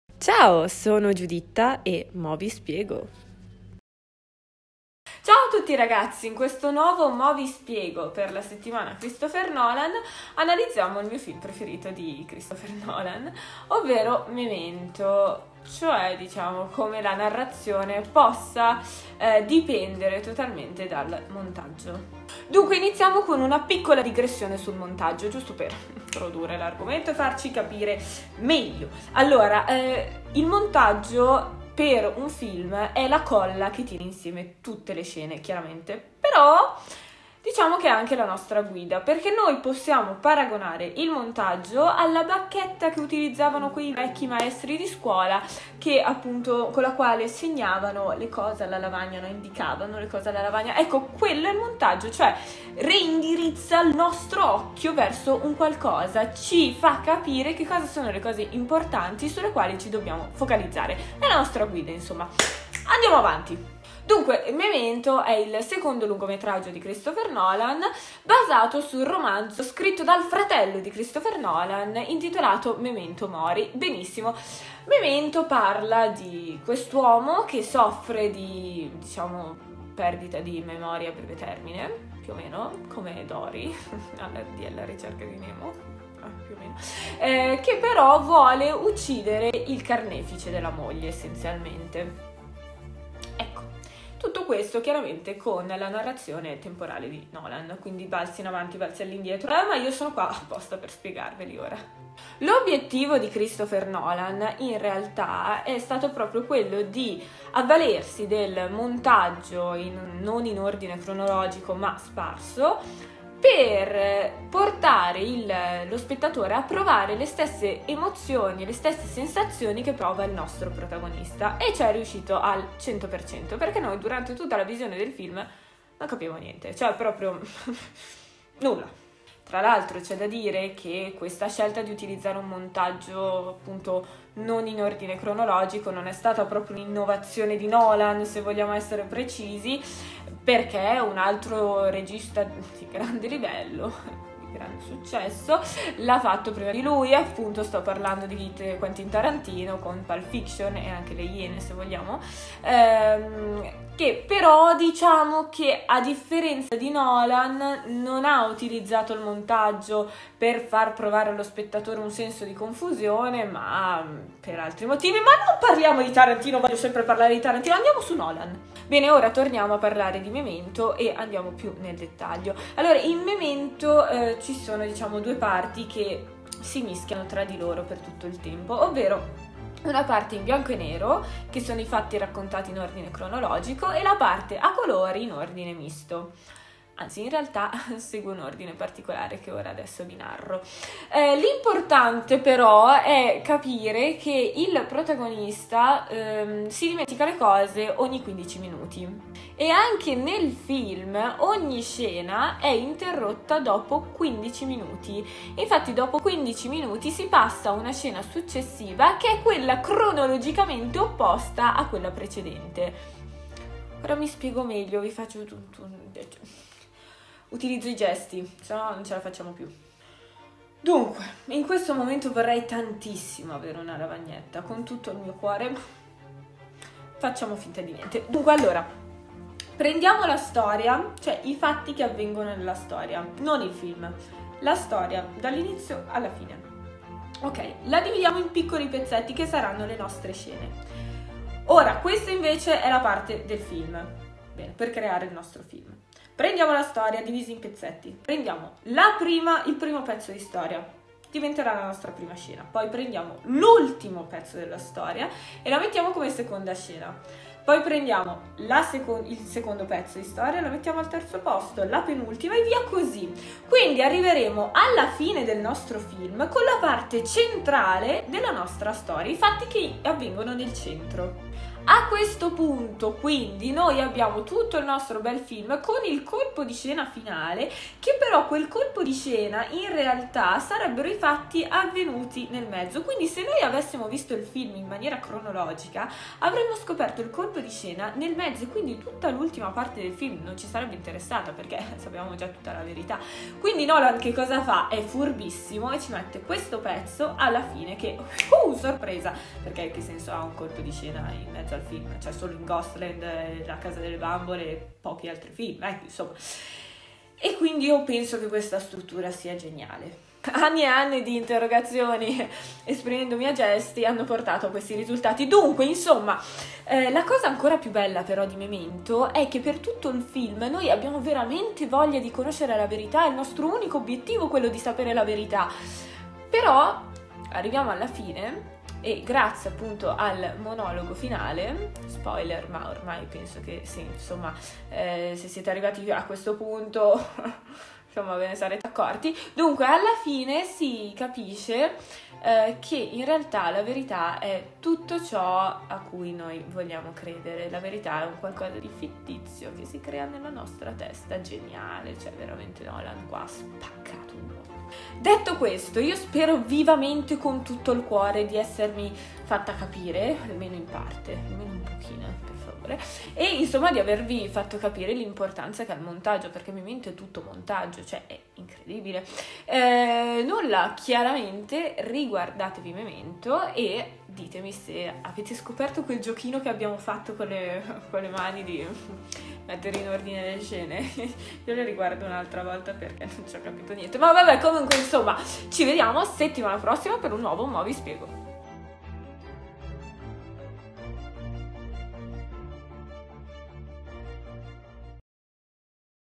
10 - Moviespiego . MEMENTO
Oggi Giuditta analizza Memento e ci spiega la tecnica di montaggio che è tipica di Nolan e come lo ha applicato al film.